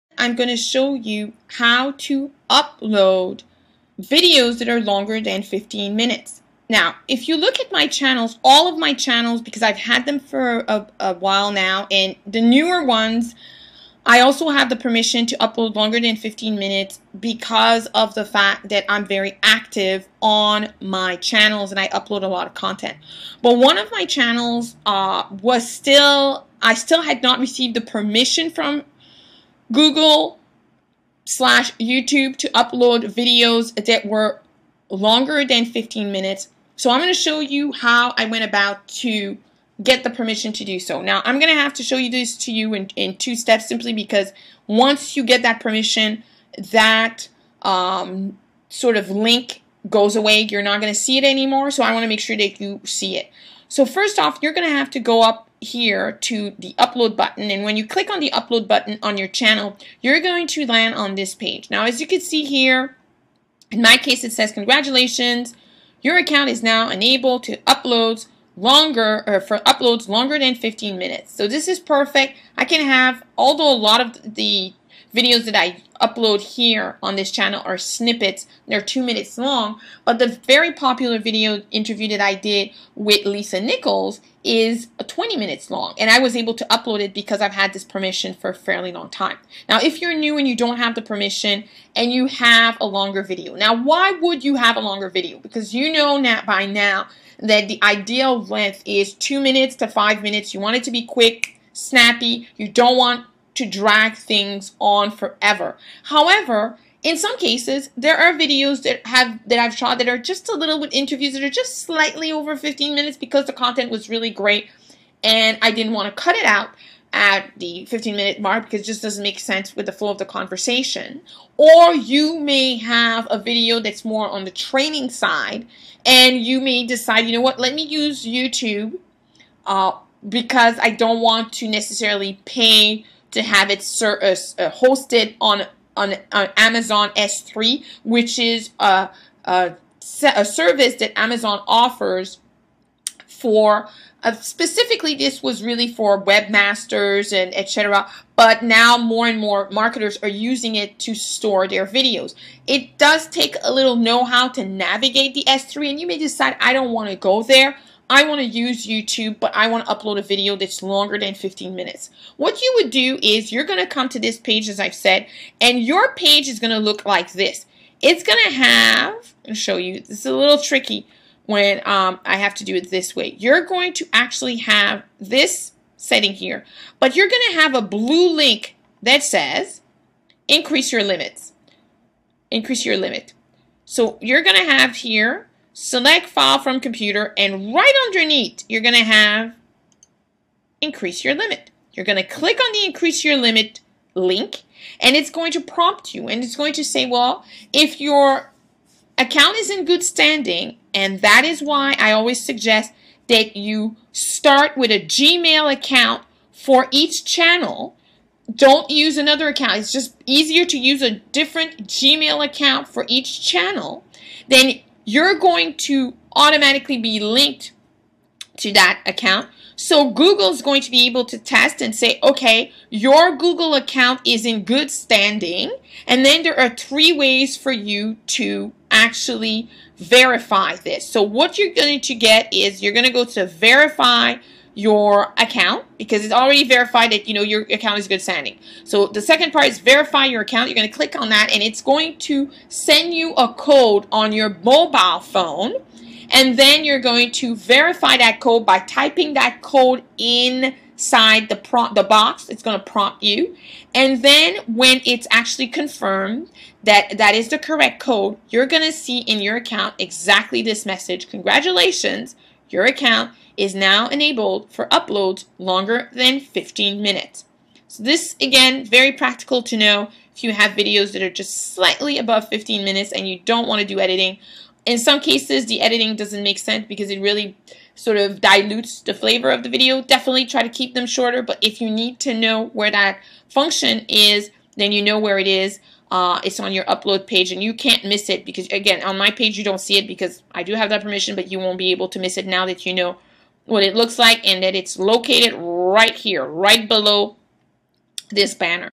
How To Upload Videos Longer Than 15 Minutes
This is an introductory video on how to upload videos longer than 15 minutes.  If you have any questions or comments please feel free to post them.  I am here to help.